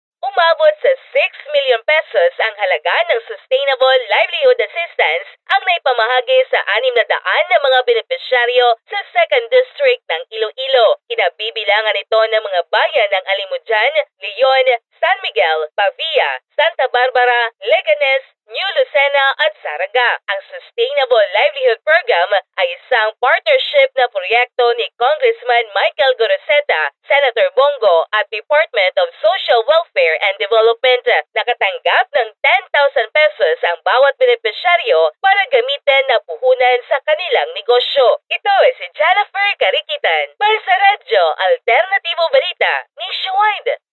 P6-M NA HALAGA NG SUSTAINABLE LIVELIHOOD PROGRAM NAIPAMAHAGI SA 600 NA MGA BENEPISYARYO SA ILOILO
P6-M NA HALAGA NG SUSTAINABLE LIVELIHOOD PROGRAM NAIPAMAHAGI SA 600 NA MGA BENEPISYARYO SA 2ND DISTRICT SA ILOILO
| via JENNIFER CARIKITAN